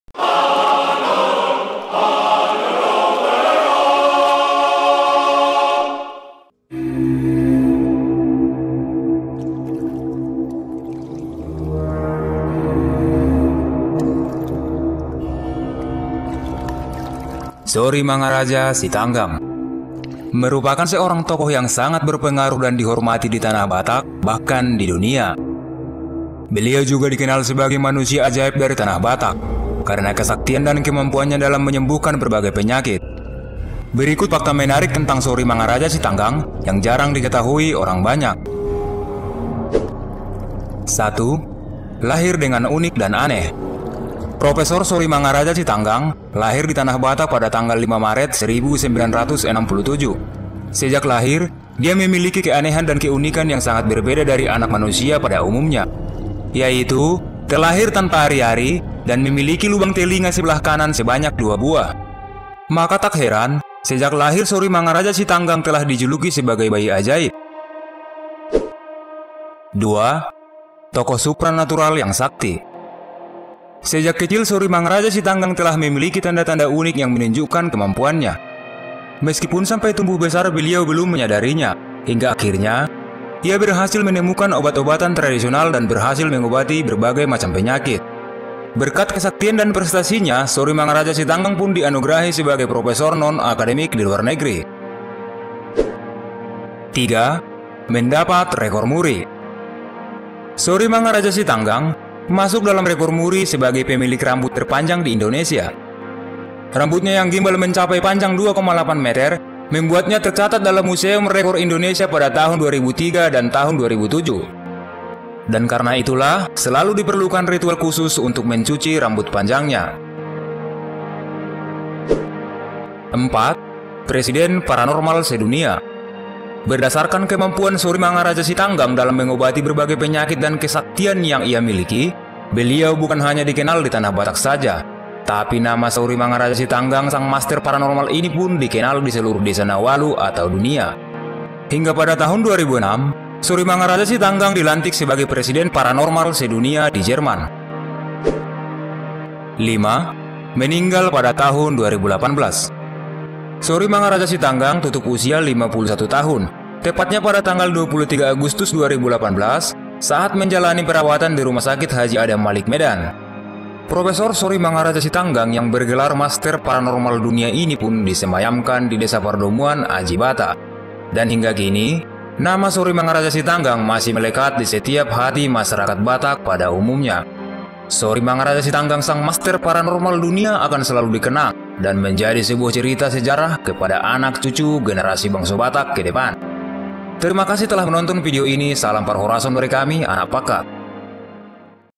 5 FAKTA TENTANG SORIMANGARAJA SITANGGANG - SI MANUSIA AJAIB YANG MENDUNIA
Sori mangaraja sitanggang Merupakan seorang tokoh yang sangat berpengaruh dan dihormati di tanah batak bahkan di dunia.
Beliau juga dikenal sebagai manusia ajaib dari tanah batak, karna kesaktian dan kemampuannya dalam menyembuhkan berbagai macam penyakit.
Sejak kecil sorimangaraja sitanggang telah memiliki tanda-tanda unik yang menunjukkan kemampuannya.
Berkat kemampuan, keberhasilan, kesaktian dan prestasi sorimangaraja sitanggang, beliau dinugerahi sebagai profesor di luar negeri.
Video ini adalah 5 fakta tentang Sorimangaraja sitanggang yang jarang diketahui orang banyak.

Digital Account:
Fanspage FB: Anak Pakkat